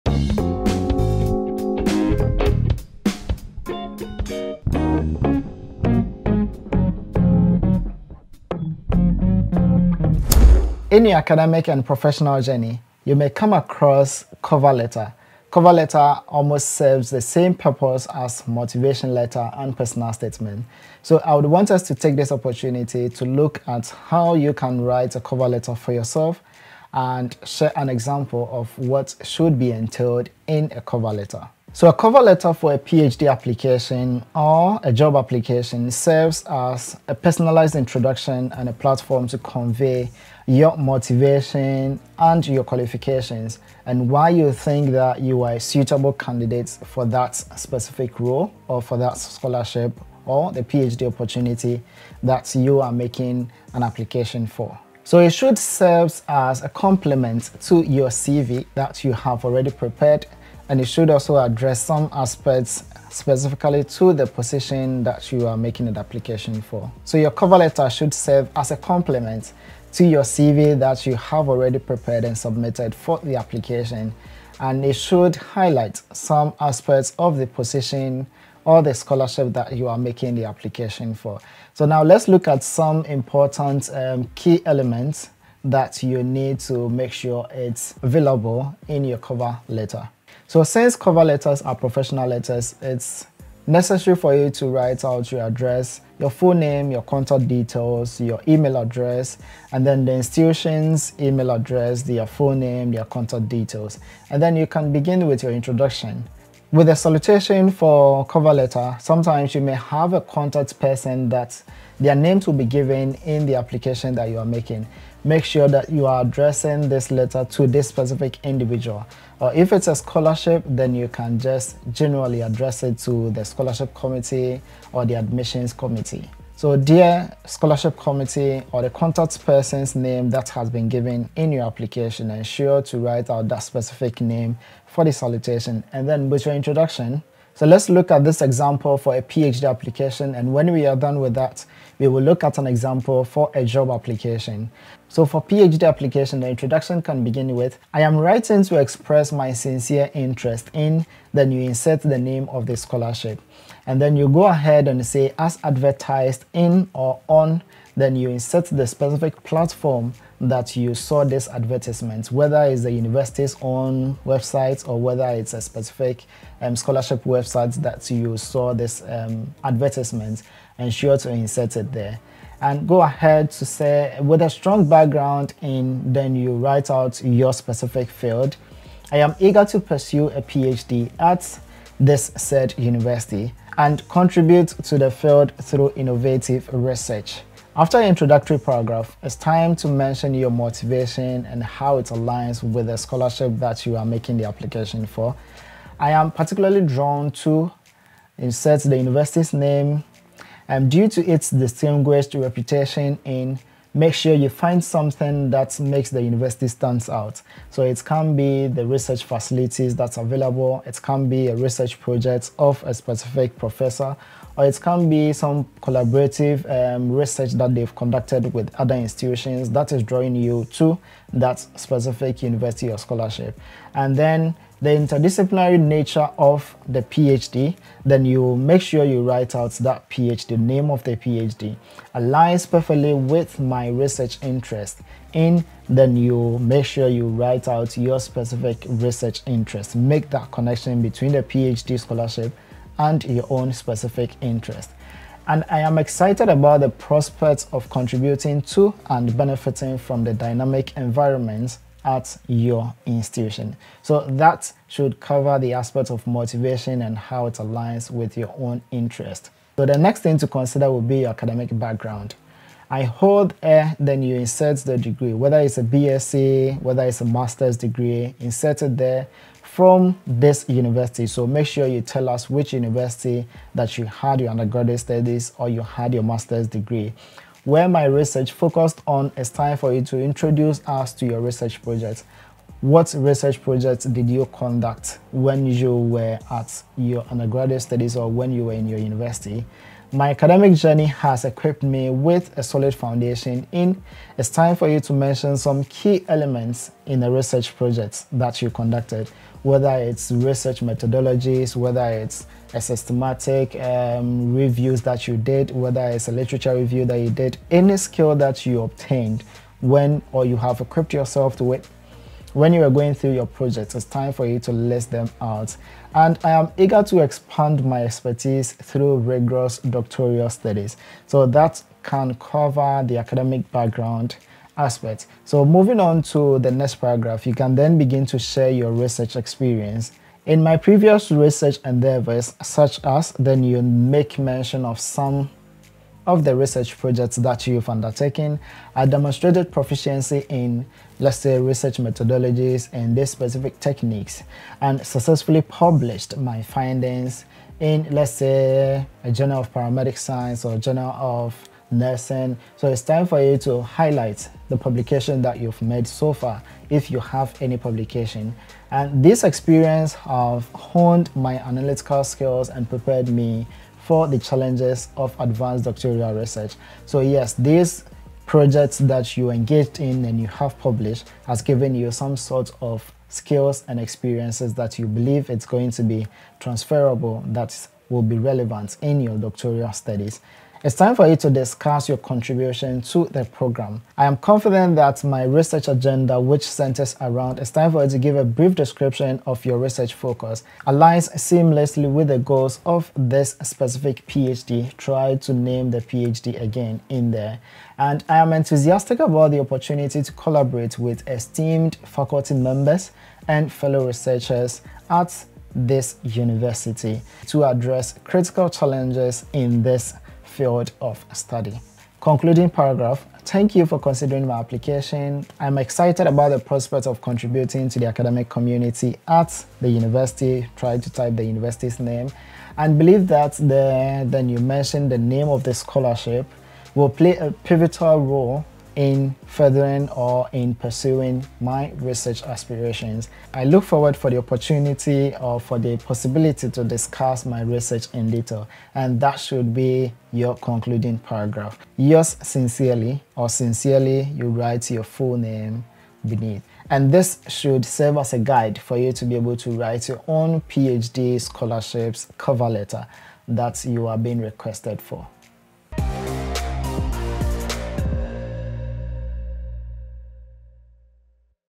How to Write an Impressive COVER LETTER!
In this video, I share the cover letter formula that will get you noticed by employers and maximises chances for scholarships . If you're struggling with your cover letter, this video is a must-watch!